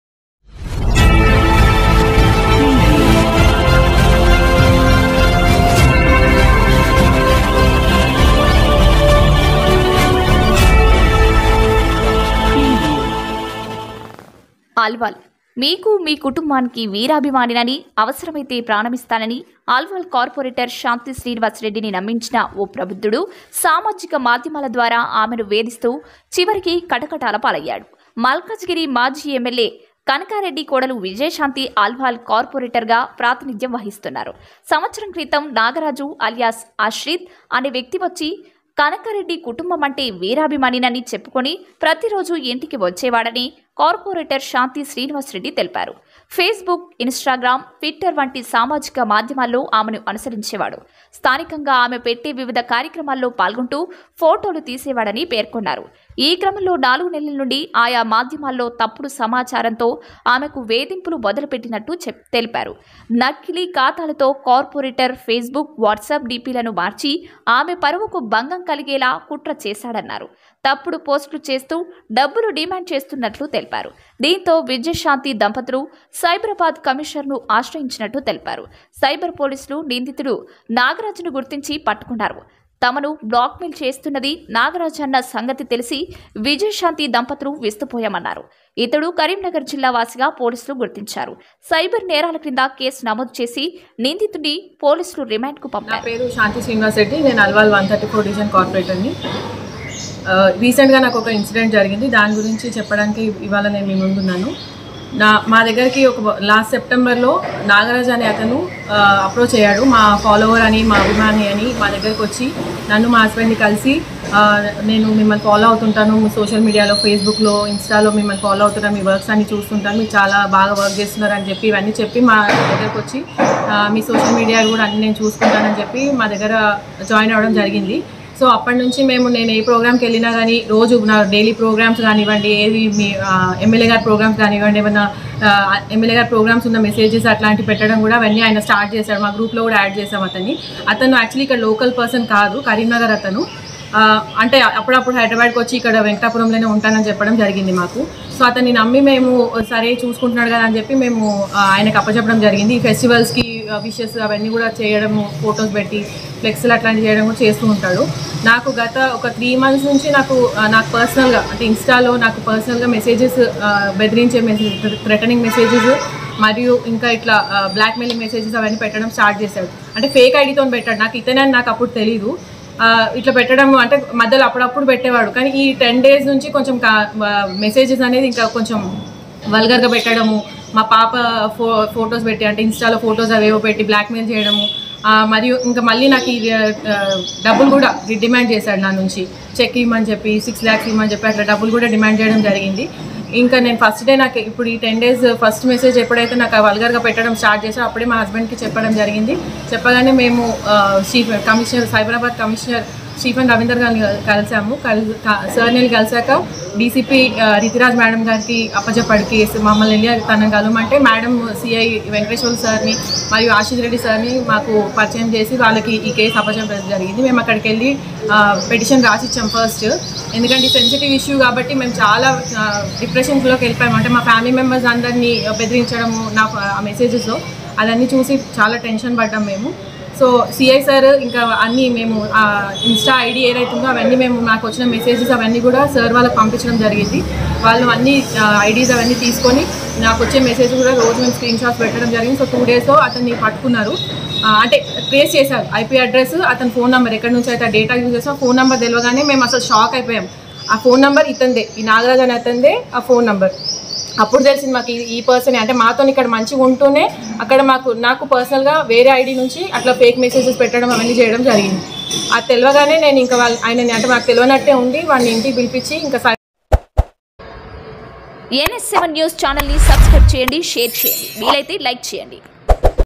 సామాజిక మాధ్యమాల ద్వారా అల్వాల్ కార్పొరేటర్ని వేధిస్తూ చివరికి కటకటాలపాయ్యాడు || NS7 NEWS
అల్వాల్

మీకు, మీ కుటుంబానికి వీరాభిమానినని, అవసర మైతే ప్రాణమిస్తానని అల్వాల్ కార్పొరేటర్ శాంతి శ్రీనివాస్ రెడ్డిని నమ్మించిన ఓ ప్రబుద్ధుడు సామాజిక మాధ్యమాల ద్వారా ఆమెను వేధిస్తూ చివరికి కటకటాలపాయ్యాడు.. మల్కాజిగిరి మాజీ ఎమ్మెల్యే కనకారెడ్డి కోడలు విజయశాంతి అల్వాల్ కార్పొరేటర్గా ప్రాతినిధ్యం వహిస్తున్నారు. సంవత్సరం క్రితం నాగరాజు అలియాస్ ఆశ్రిత్(28) అనే వ్యక్తి వచ్చి కనకారెడ్డి కుటుంబమంటే వీరాభిమా నమని చెప్పుకొని ప్రతిరోజూ ఇంటికి వచ్చేవాడని కార్పొరేటర్ శాంతి శ్రీనివాస్ రెడ్డి తెలిపారు. ఫేస్ బుక్, ఇన్స్టాగ్రాం, ట్విట్టర్ వంటి సామాజిక మాధ్య మాల్లో ఆమెను అనుసరించేవాడు. స్థానికంగా ఆమె పట్టే వివిధ కార్యక్రమాల్లో పాల్గొంటూ ఫొటోలు
తీసేవాడని పేర్కొన్నారు. ఈ క్రమంలో నాలుగు నెలల నుంచి ఆయా మాధ్యమాల్లో తప్పుడు సమాచా రంతో ఆమెకు వేధింపులు మొదలు పెట్టినట్టు తెలిపారు. నకిలీ ఖాతాతో కార్పొరేటర్ ఫేస్బుక్, వాట్సాప్ డీపీలను మార్చి, ఆమె పరువుకు భంగం కలి గేలా కుట్ర చేశాడన్నారు. తప్పుడు పోస్టులు చేస్తూ డబ్బులు డిమాండ్ చేస్తున్నట్లు తెలిపారు. దీంతో విజయ శాంతి దంపతులు సైబరాబాద్ కమిషనర్ను ఆశ్రయించినట్లు తెలిపారు. సైబర్ పోలీసులు నిందితుడు నాగరా జును గుర్తించి పట్టుకున్నారు. తమను బ్లాక్మెయిల్ చేస్తున్నది నాగరాజన్న సంగతి తెలిసి విజయశాంతి -దంపతులు విస్తుపోయామన్నరు. అతడు కరీంనగర్ జిల్లా వాసిగా పోలీసులు గుర్తించారు. సైబర్ నేరాల కింద కేసులు నమోదు చేసి, నిందితుడిని  పోలీసులు రిమాండ్ కు పంపారు.

బైట్..శాంతి శ్రీనివాస్ రెడ్డి...అల్వాల్ కార్పొరేటర్